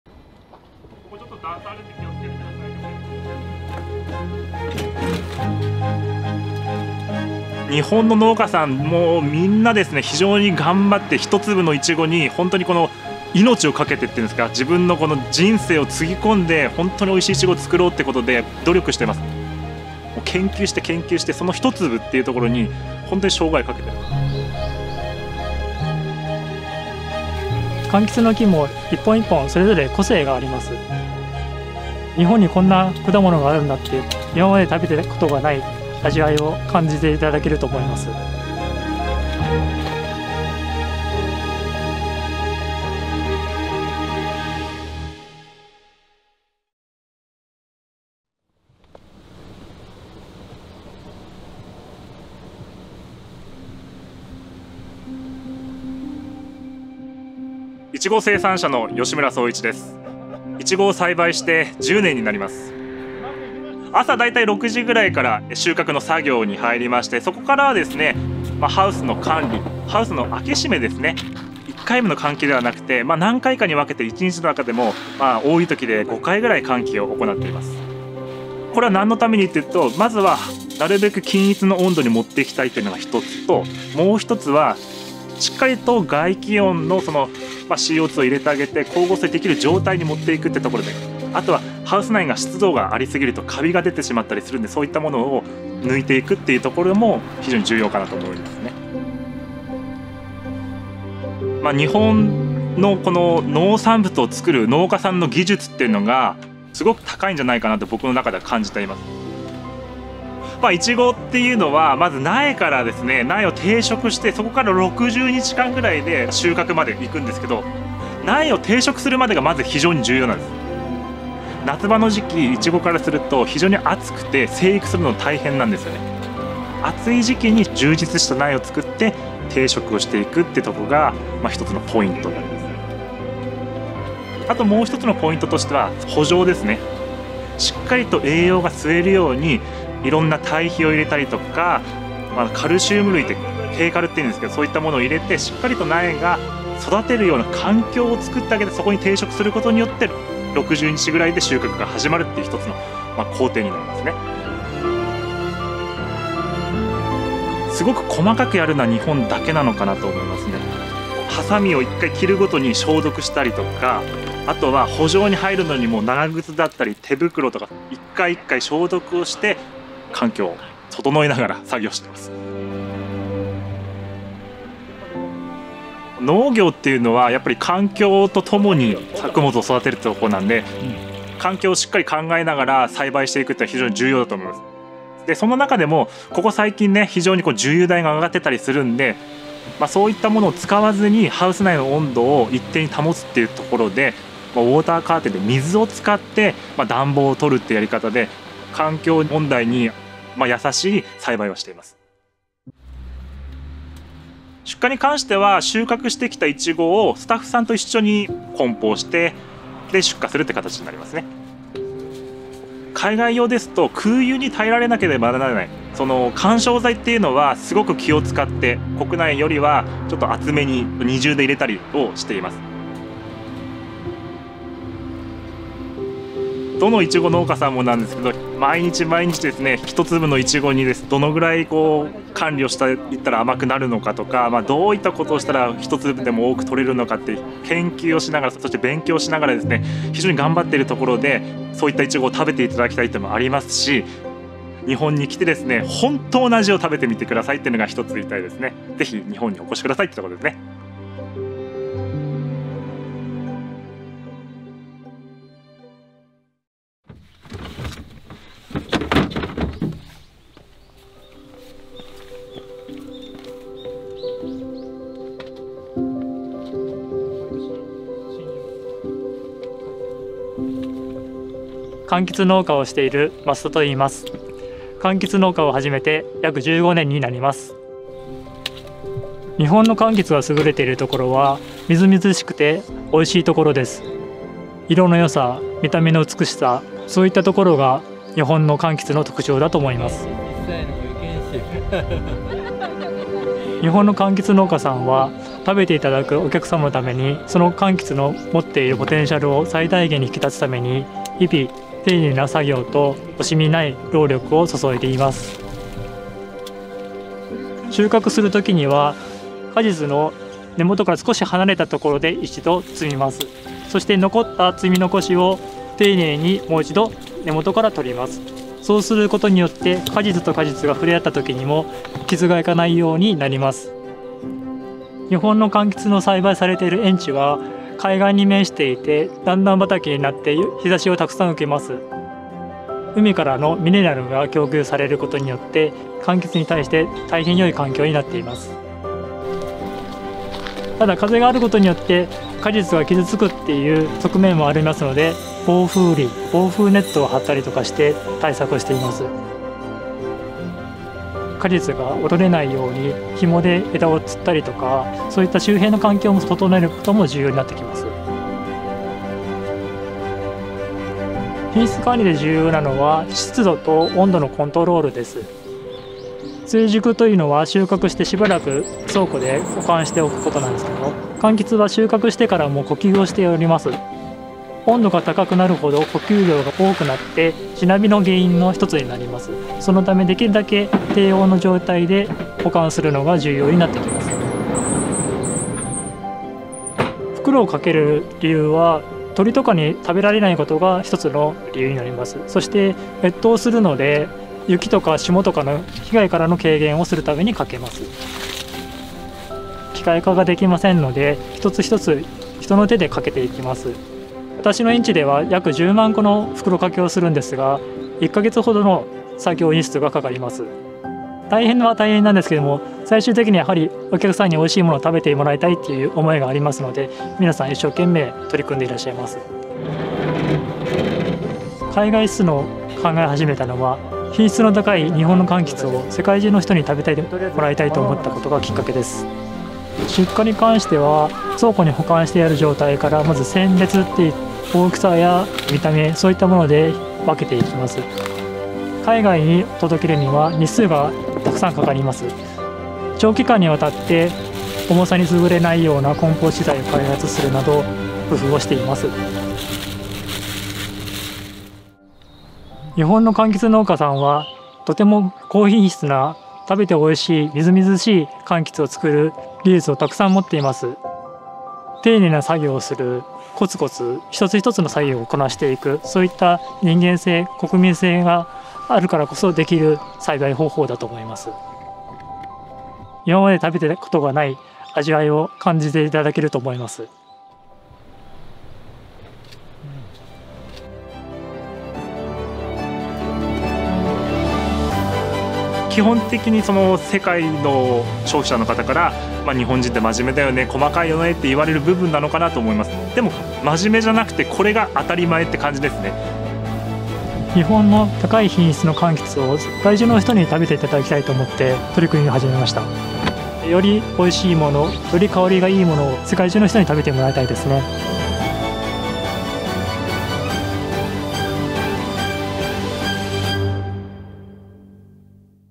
The Interview with Japanese Fruits Farmers (Thai)/ สัมภาษณ์เกษตรกรชาวสวนผลไม้ในประเทศญี่ปุ่น
สัมภาษณ์เกษตรกรชาวสวนผลไม้ในประเทศญี่ปุ่น